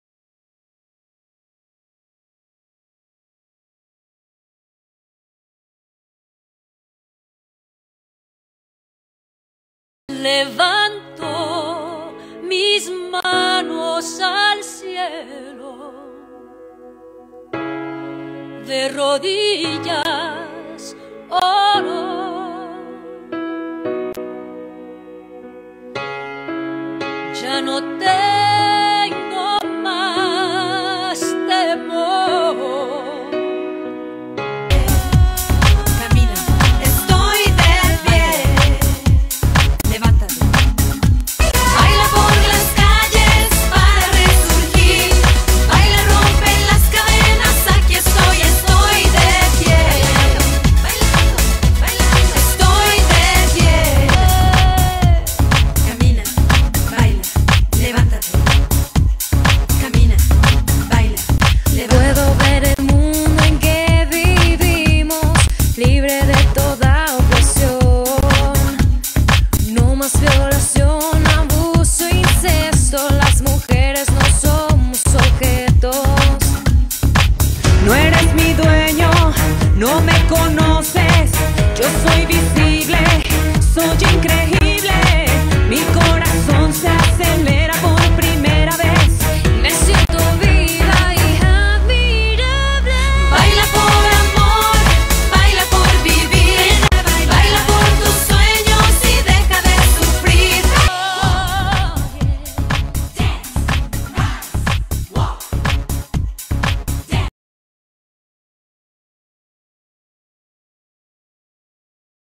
1 BILLON DE PIE
El 14 de Febrero miles de personas alrededor del mundo cantaran y bailaran contra la violencia a mujeres y niñas del mundo
En Santiago, varios grupos estámos planeando eventos alrededor de esta fecha* buscando promover una cultura respetuosa, no opresiva y no violenta hacia las mujeres
blogger: